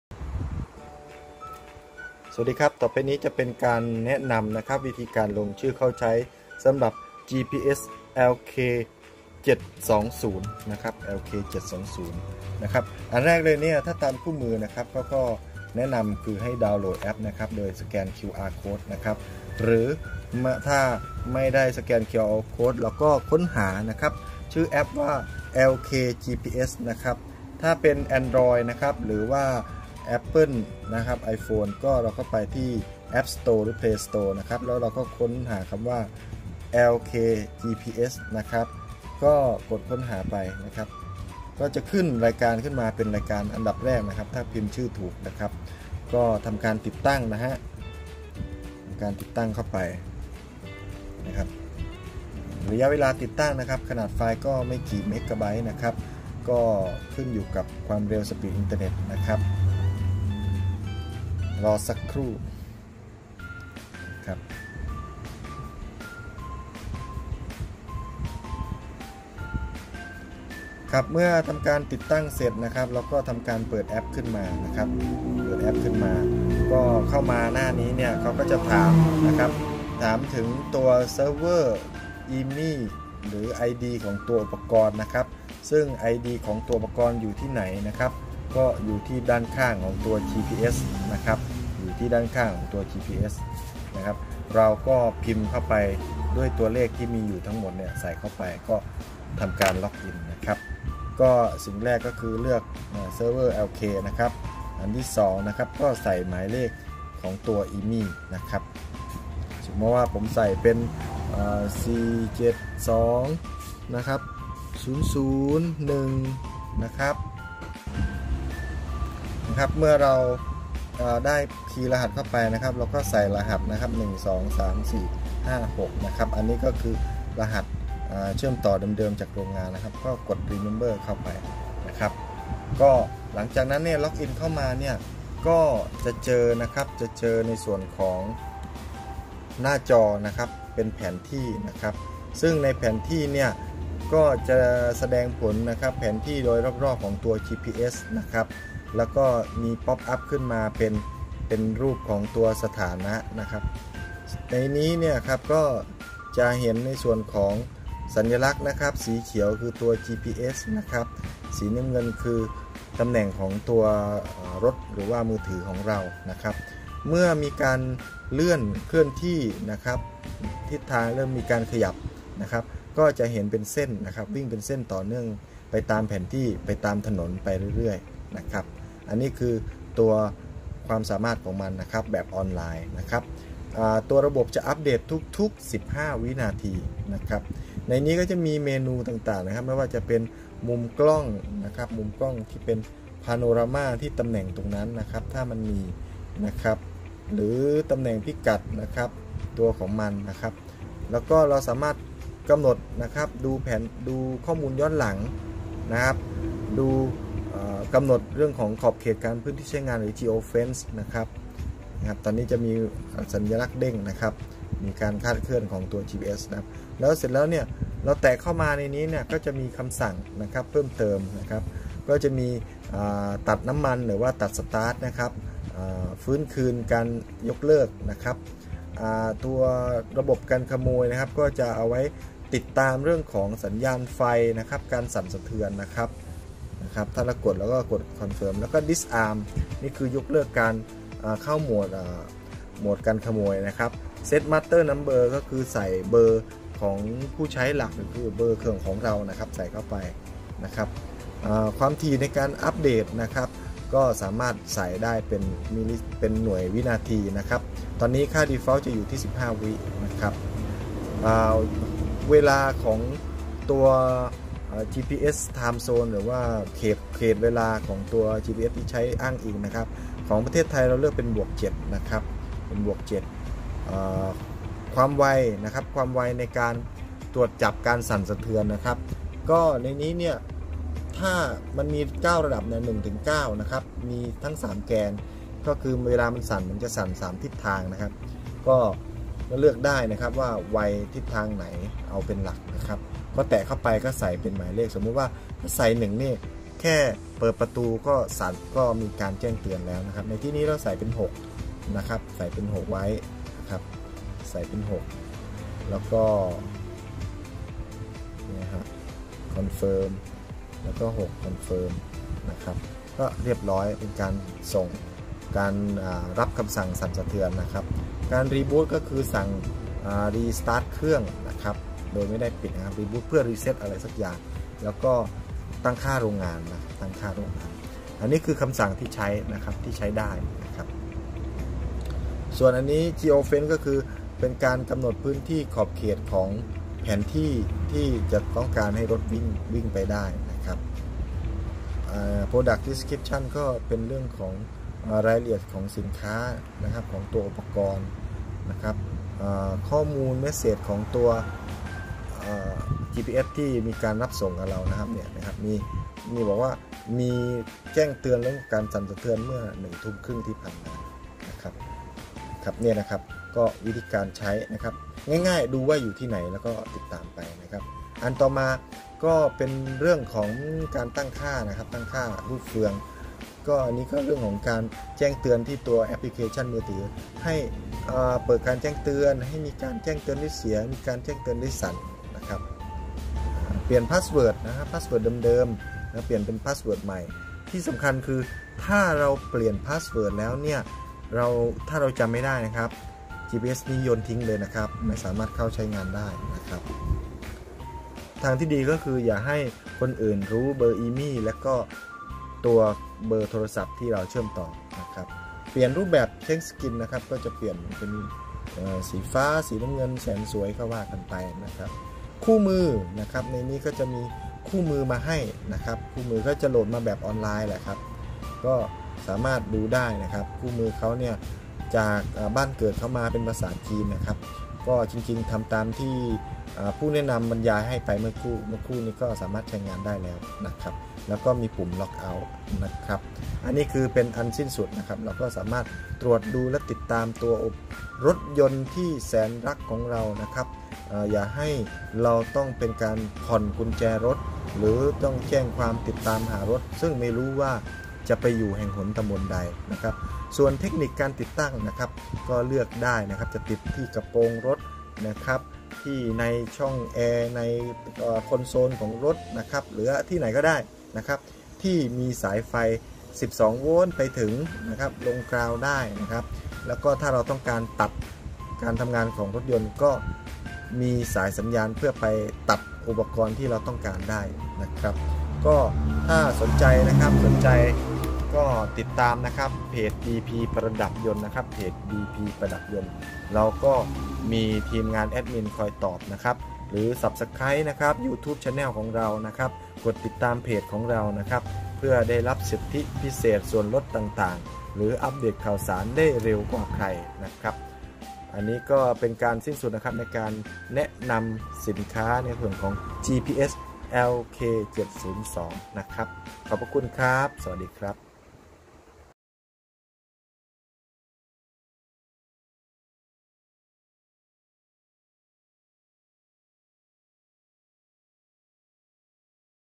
วิธีการใช้งาน โปรแกรมติดตาม จีพีเอส ติดตาม LKGPS DPAUTOSHOP
การใช้งาน ขั้นตอนการเข้าใช้ คำสั่งภายใน